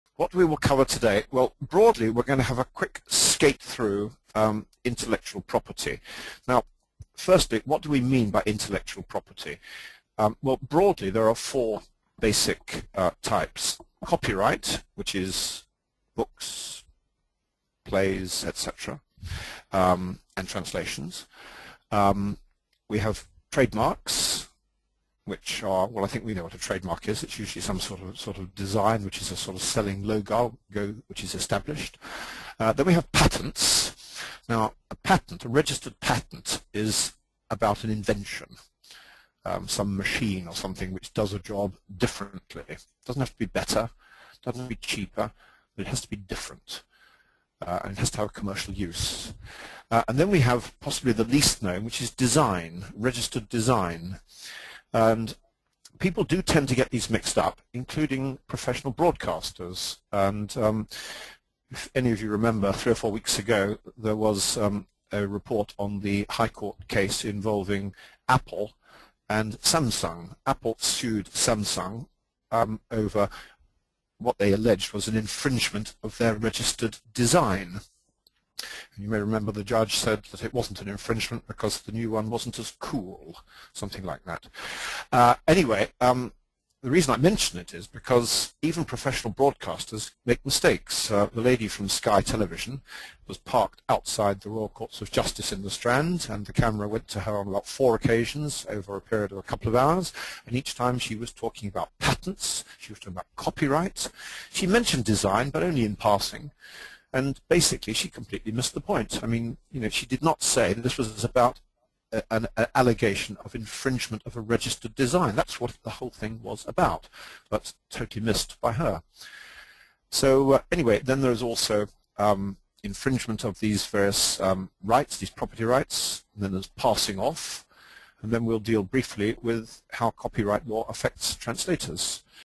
Intellectual Property for translators
WHO SHOULD WATCH IT

Translators and interpreters interested in intellectual property law and legal translation.

WHAT ARE THE BENEFITS TO YOU

This video will increase your knowledge about intellectual property for translators.